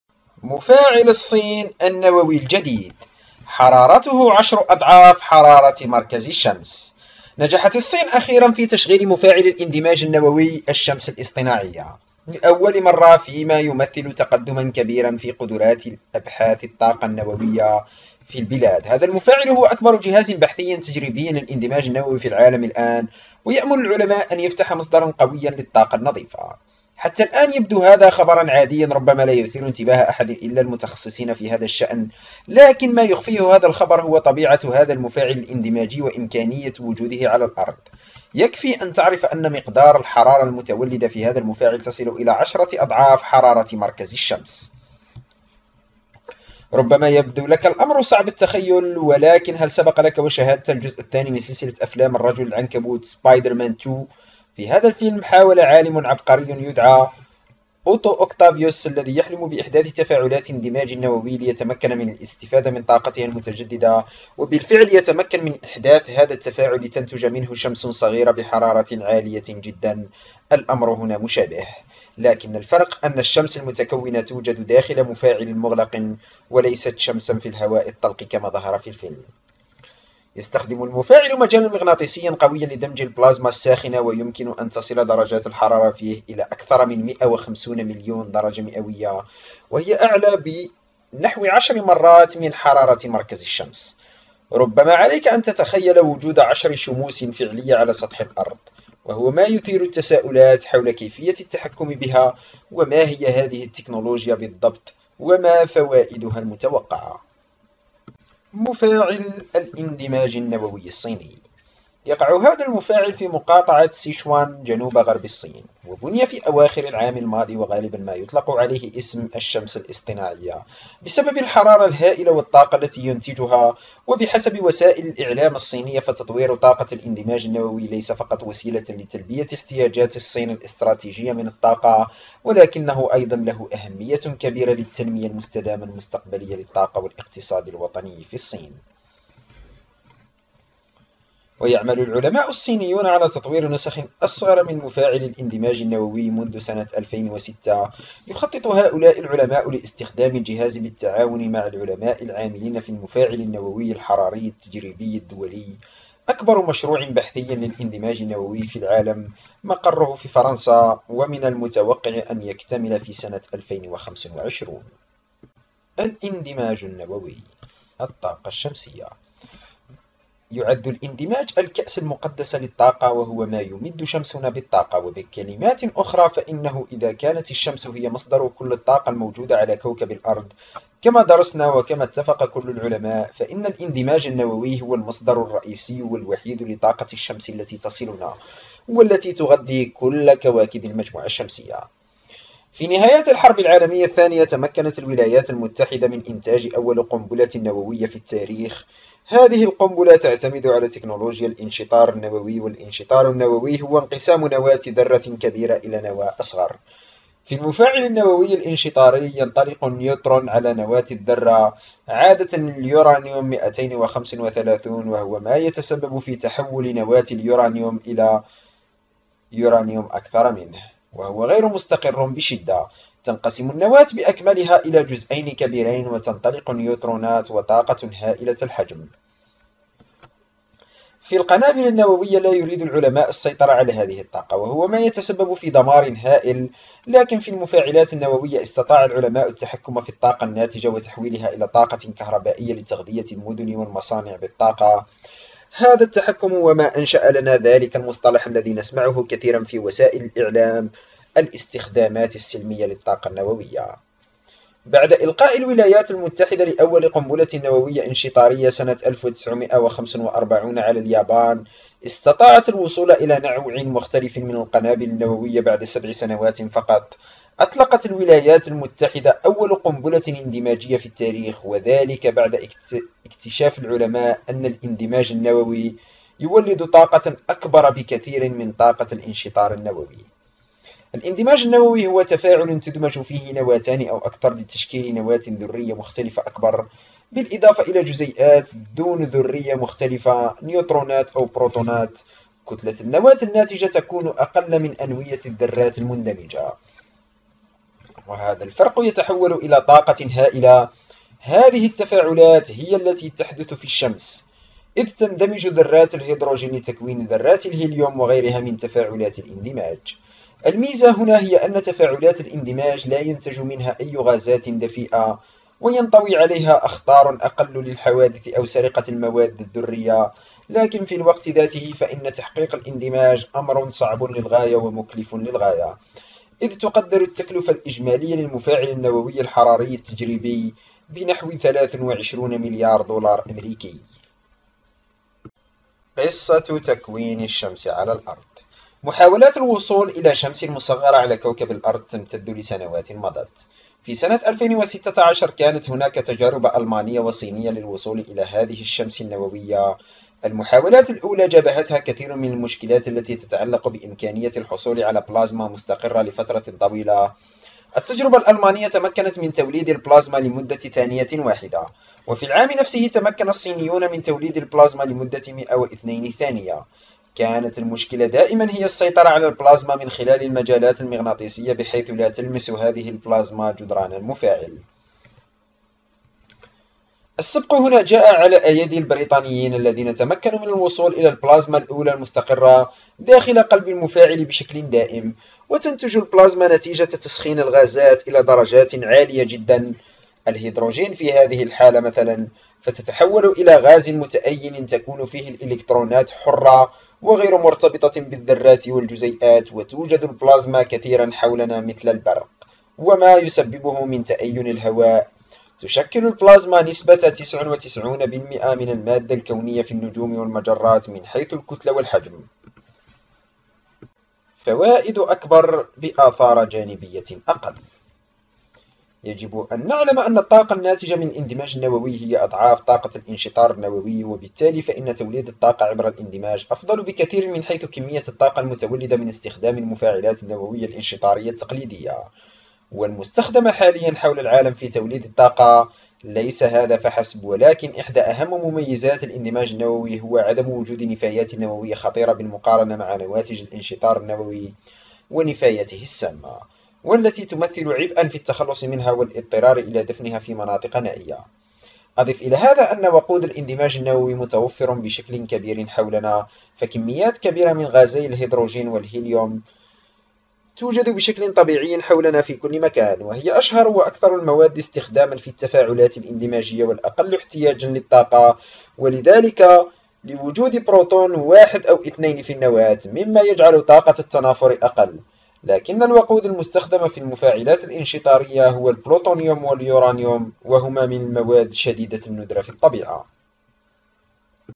مفاعل الصين النووي الجديد .. حرارته 10 أضعاف حرارة مركز الشمس!
نجحت الصين أخيرًا في تشغيل مفاعل الاندماج النووي «الشمس الاصطناعية» لأول مرة، فيما يمثل تقدمًا كبيرًا في قدرات أبحاث الطاقة النووية في البلاد. هذا المفاعل هو أكبر جهاز بحثي تجريبي للاندماج النووي في العالم الآن، ويأمل العلماء في أن يفتح مصدرًا قويًا للطاقة النظيفة.

China has finally launched the "artificial sun" nuclear fusion reactor for the first time, marking a major advance in the country's nuclear energy research capabilities. This reactor is now the world's largest experimental nuclear fusion research device, and scientists hope it opens a powerful source of clean energy.